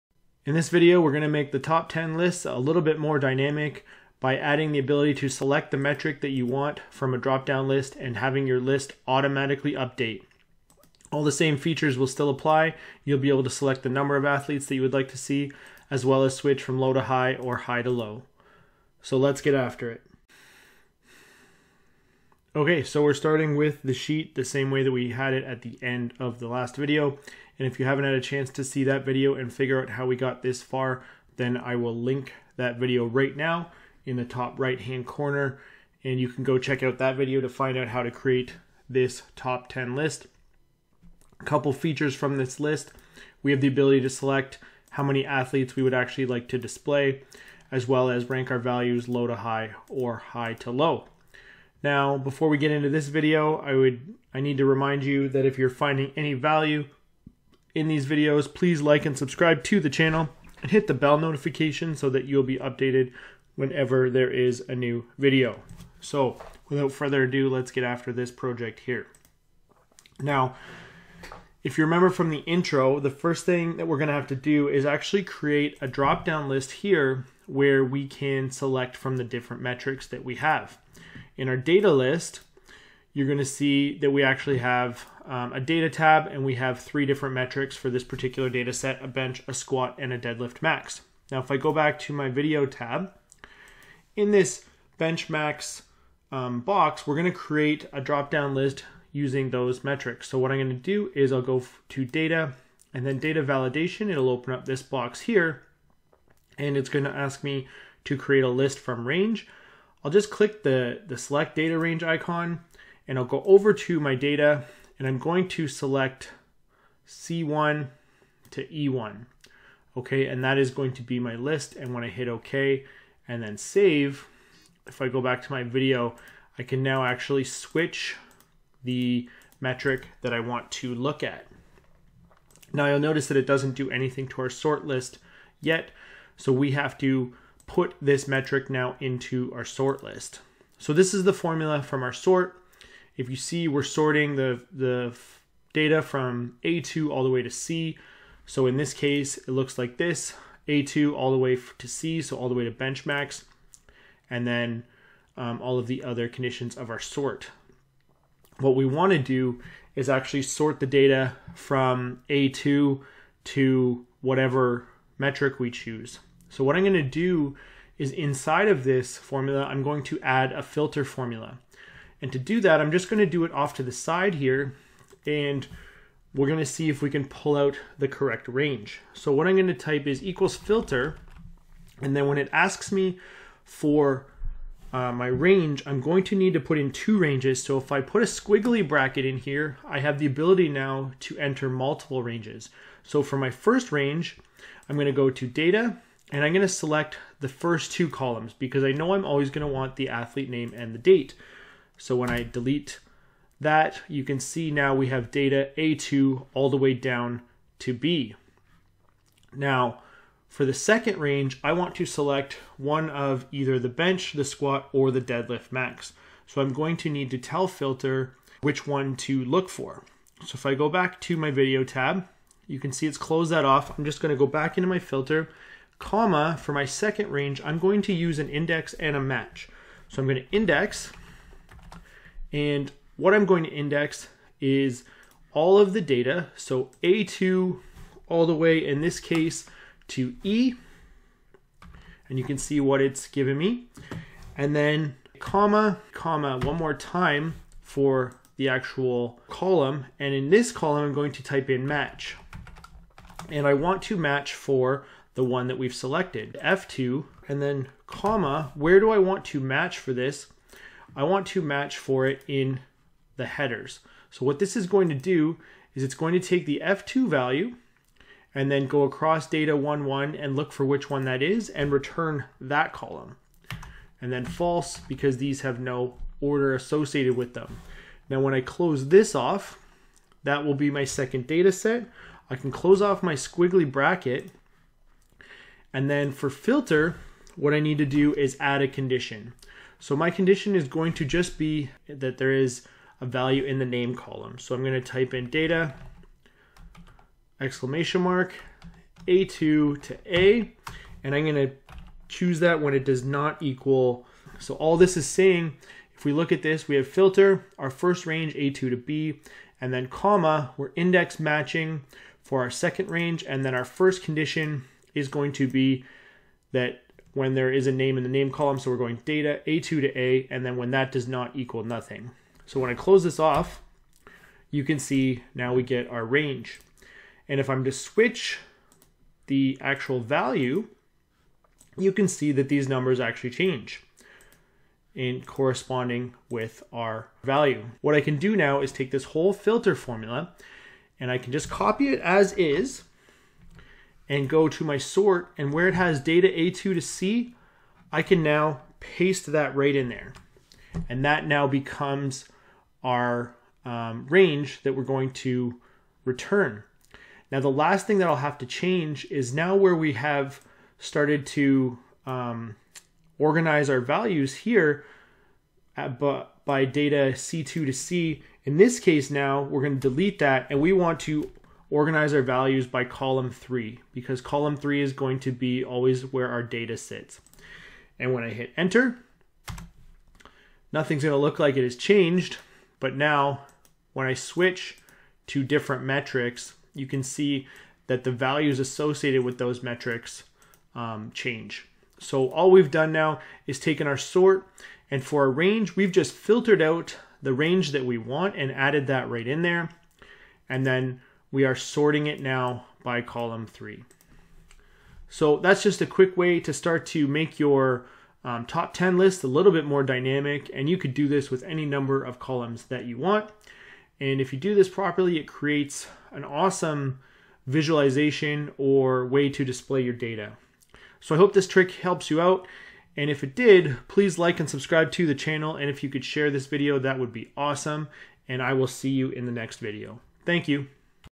Even More Dynamic Top 10 List in Google Sheets
In this video we add the ability to switch the metrics that you would like displayed in your top 10 list. This is a powerful feature.

►Strength Coach Guide to Excel
►Strength Coach Playbook
►Principles and Practice of Resistance Training
►High Performance Training for Sport